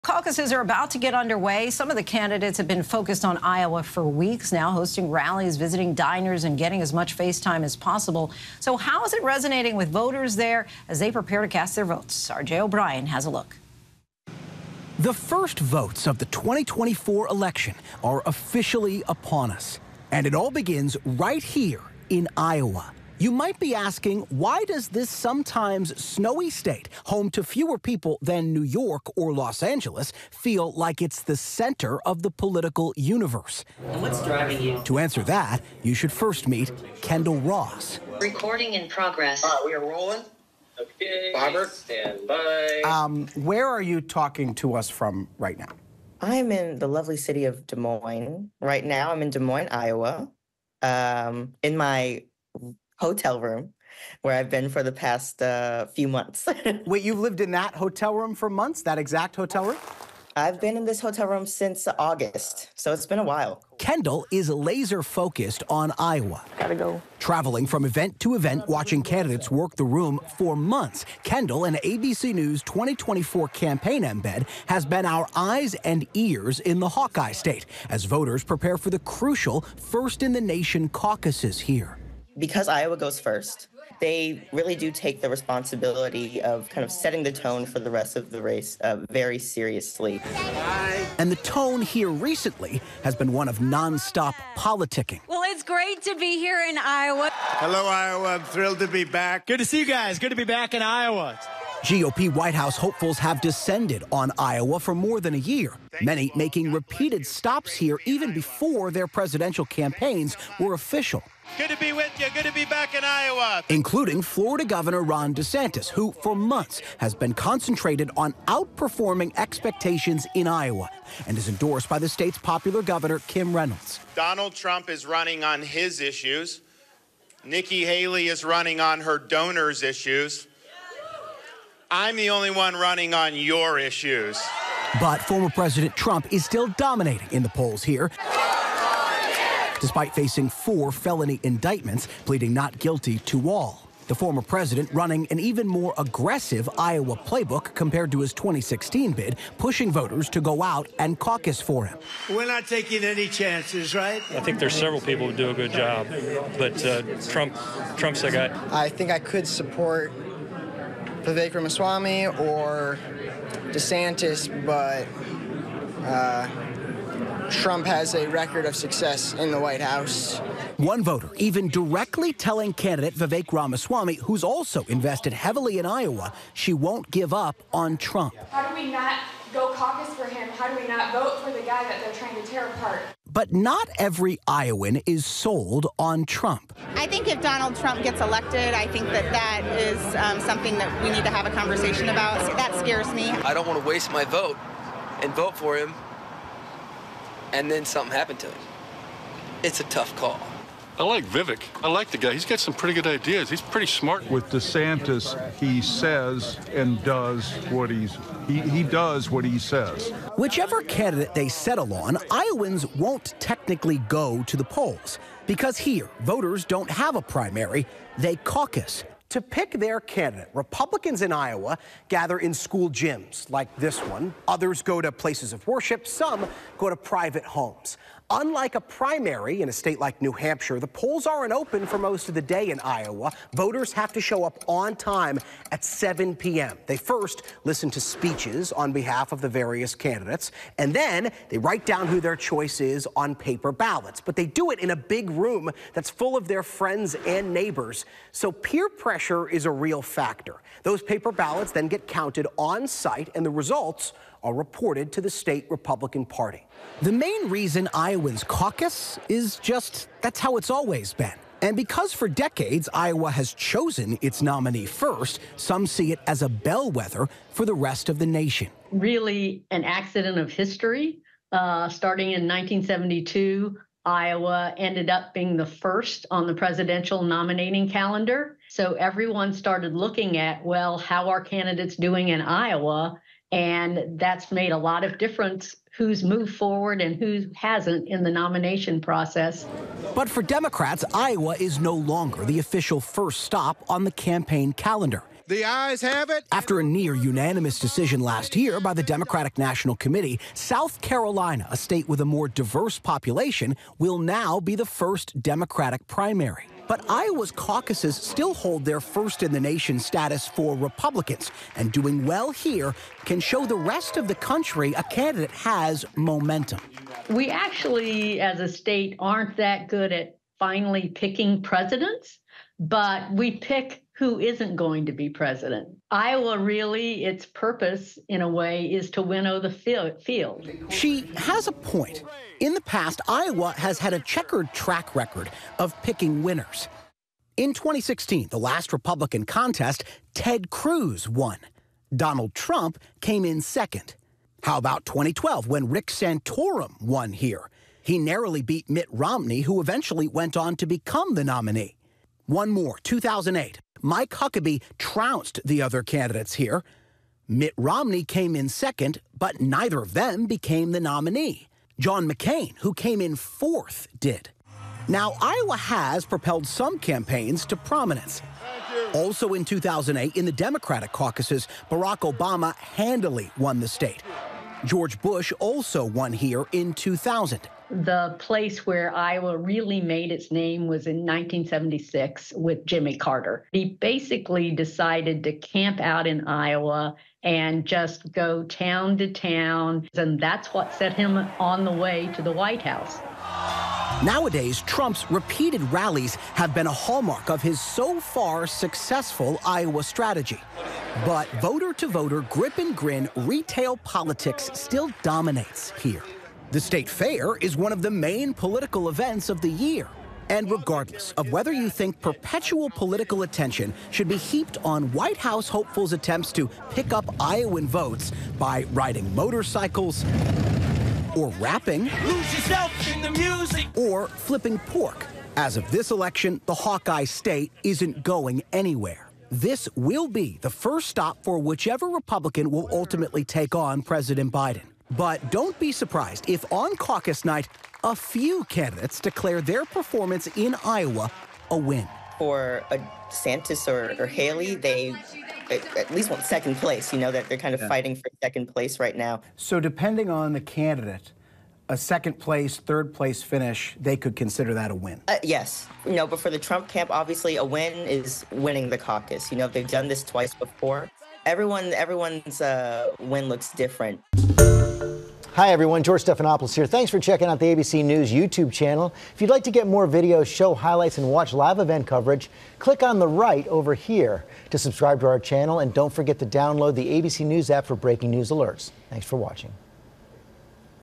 The final sprint to Iowa as the 1st 2024 election votes begin
ABC News’ Jay O’Brien reports on how the Iowa caucus impacts the GOP race to the White House.